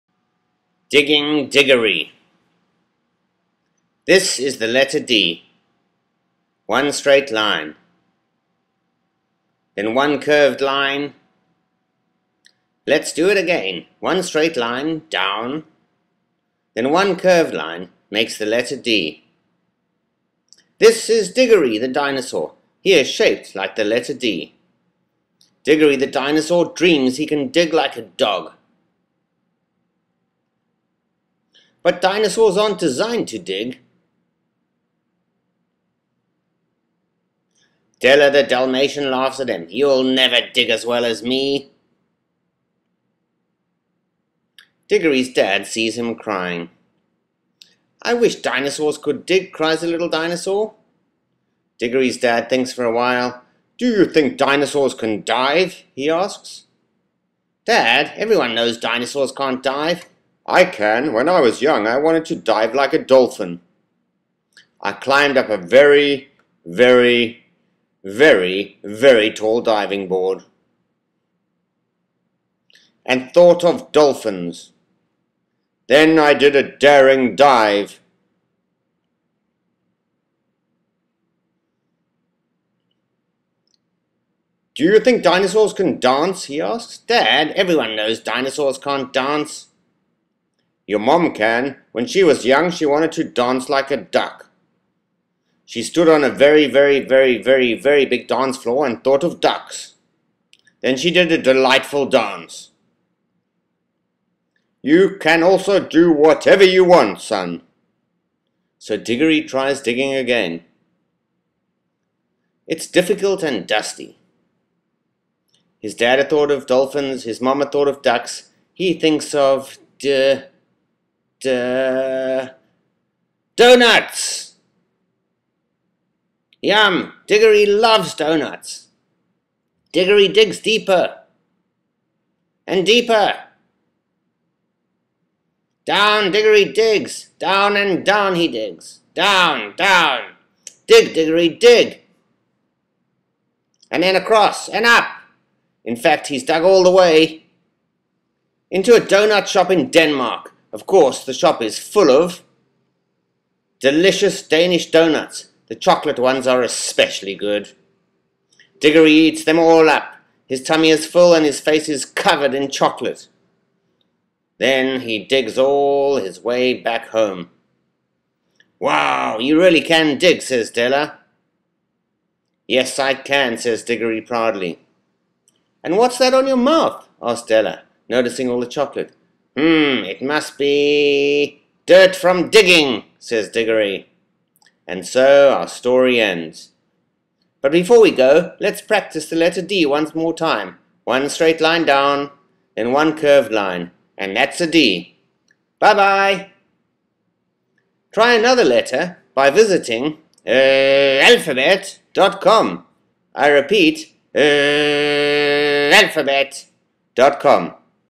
The alphabet for kids. Letter D: Digging Diggory
Kids can learn each letter of the alphabet through a fun and funny story. Learn the letter D. Core story components are: Letter Shape (what it looks like, multiple highlighted exposures within story text, repetition), Letter Sound: (same sound repeated in different words, focus on single (unblended) sounds, repetition), Alliteration (using familiar words with clear letter sounds). ABC Alphabet.